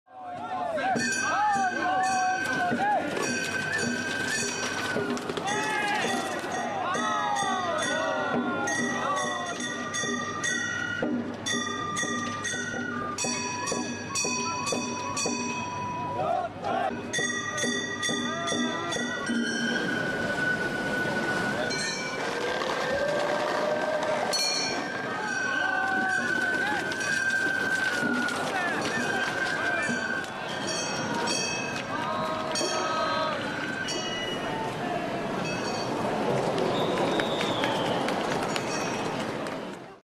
Colourful floats wow crowds at Kyoto's Gion festival
Spectators braved sweltering heat to watch a series of colourful floats at the annual Gion festival on Tuesday.